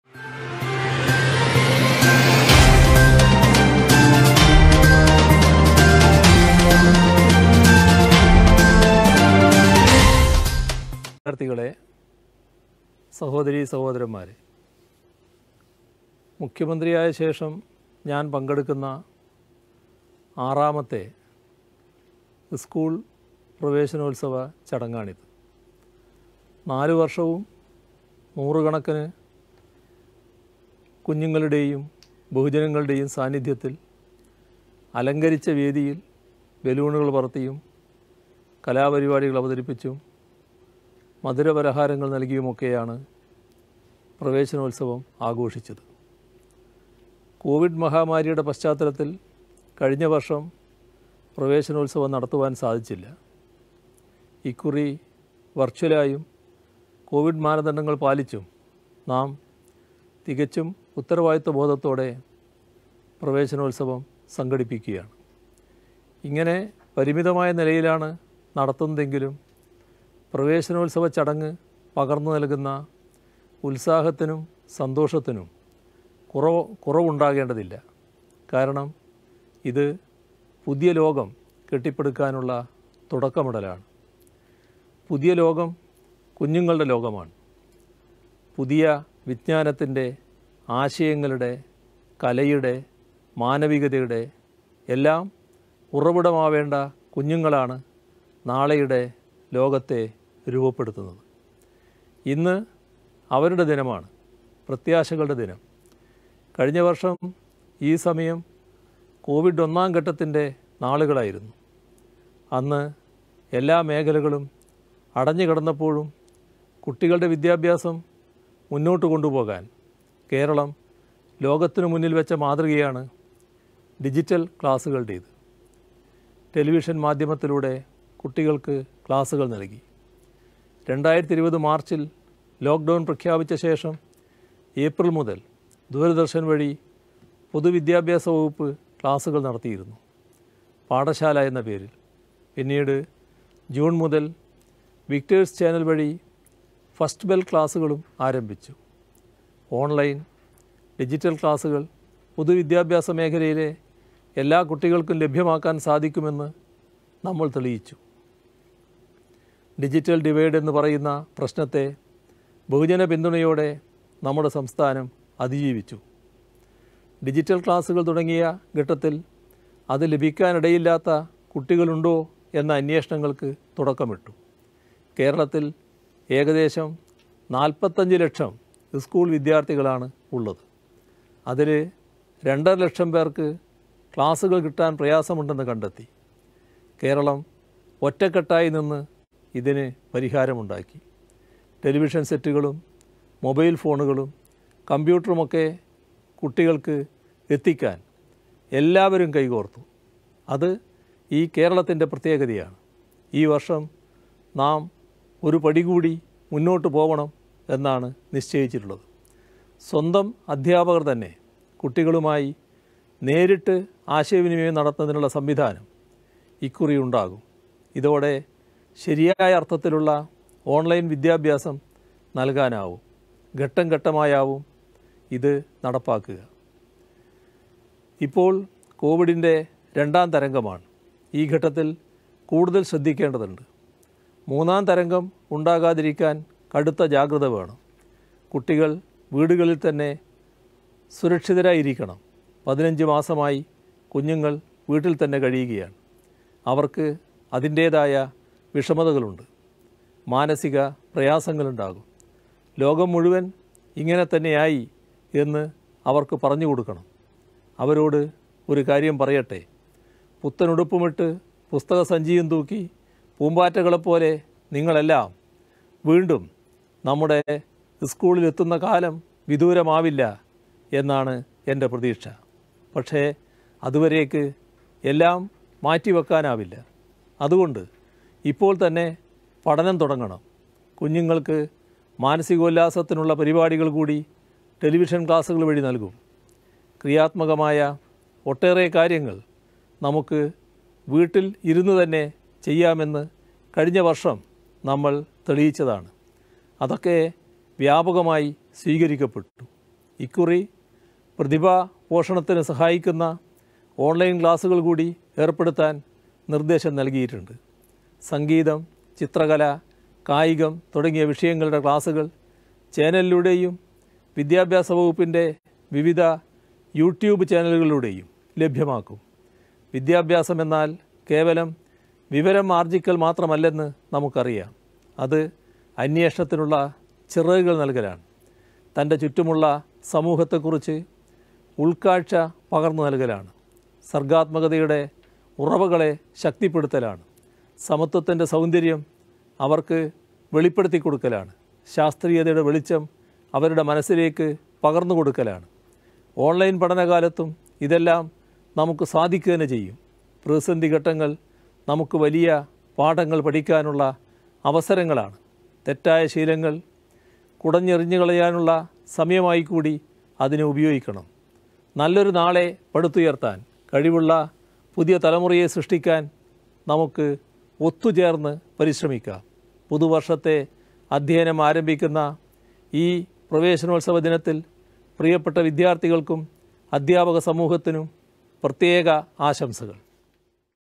കുട്ടികള്‍ നാളയെ നയിക്കേണ്ടവര്‍ : മുഖ്യമന്ത്രി പിണറായി വിജയന്‍
വിജ്ഞാനത്തിന്‍റെ, ആശയങ്ങളുടെ, കലയുടെ, മാനവികതയുടെ എല്ലാം ഉറവിടമകേണ്ട കുഞ്ഞുങ്ങള്‍ ആണ് നാളെയുടെ ലോകത്തെ രൂപപ്പെടുത്തേണ്ടത് എന്ന് മുഖ്യമന്ത്രി പിണറായി വിജയന്‍ പറഞ്ഞു. ഈ വര്‍ഷത്തെ സ്കൂള്‍ പ്രവേശനോത്സവം വെര്‍ച്വലായി ഉദ്ഘടാനം ചെയ്തു സംസാരിക്കുകയായിരുന്നു മുഖ്യമന്ത്രി പിണറായി വിജയന്‍. 45 ലക്ഷത്തോളം കുട്ടികൾ വീടുകളിൽ ഇരുന്ന് പ്രവേശനോത്സവത്തിൽ പങ്കാളികളാകുന്നത്